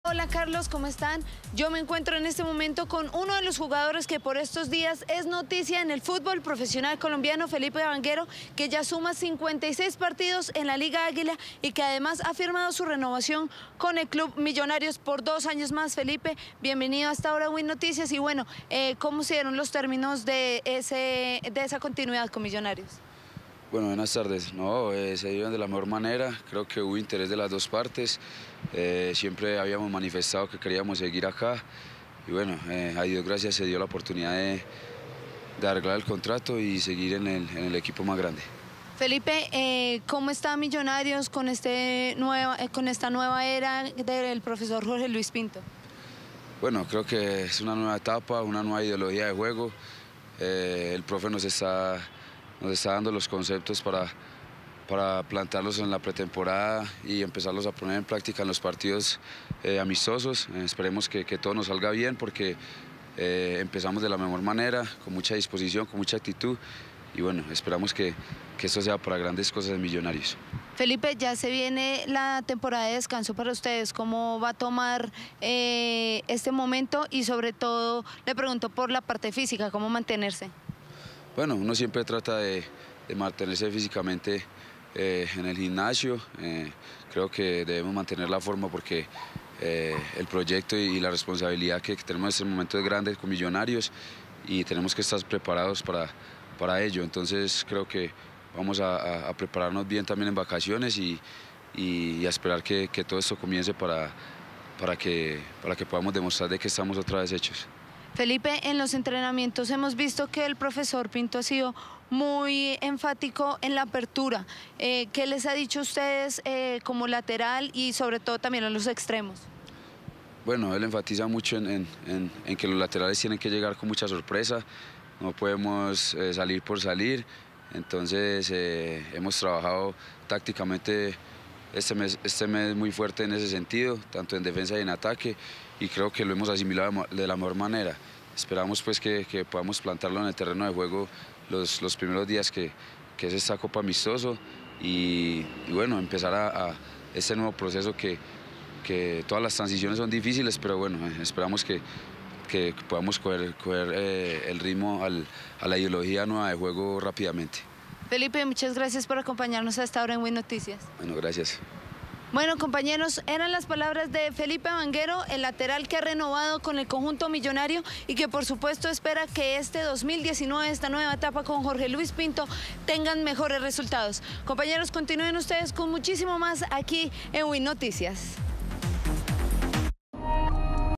Felipe Banguero: Con Pinto esperamos grandes cosas para Millonarios
El lateral izquierdo renovó por 2 años más con los 'embajadores'.